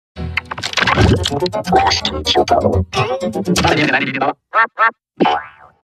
(REQUESTED) Klasky Csupo in Otuavocoding
Requested By @PMW2K1

1st: Gradient Map

0: Yellow
1: Pink
2: Black
3: White

Audio:

1st: IL Vocodex - Autovocoding

Modulator Pitch Shift: Maximum